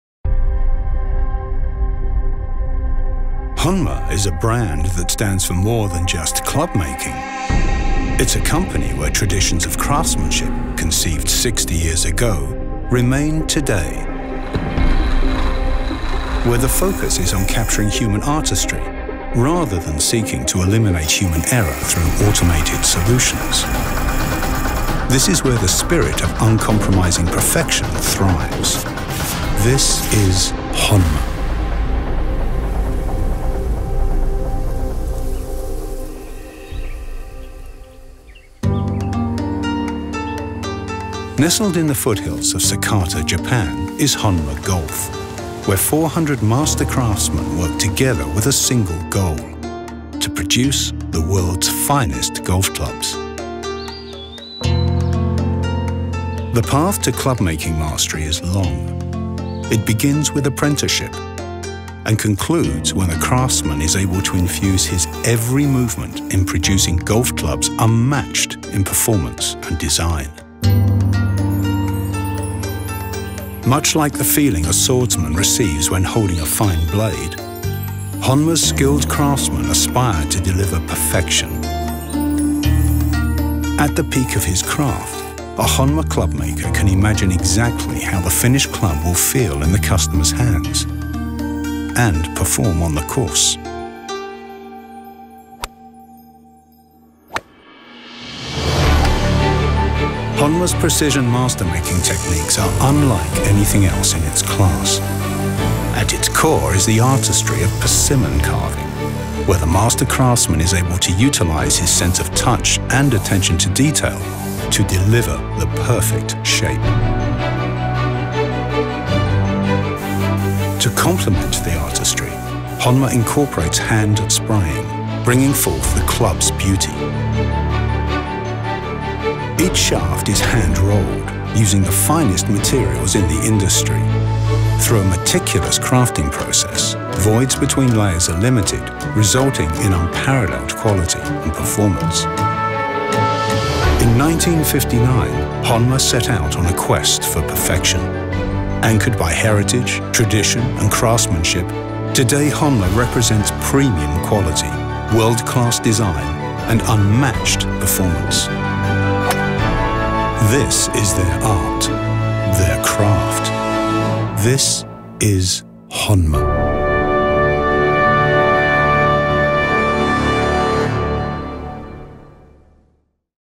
The Home of Precision Craftsmanship
Since 1959, Honma Golf has been making some of golf’s most coveted clubs. In the foothills of Sakata, Japan, more than 400 skilled craftsmen combine superior materials with unbeatable craftsmanship to produce perfection. Perfection in precision, feel, and performance. This is the art. The craft. This is Honma.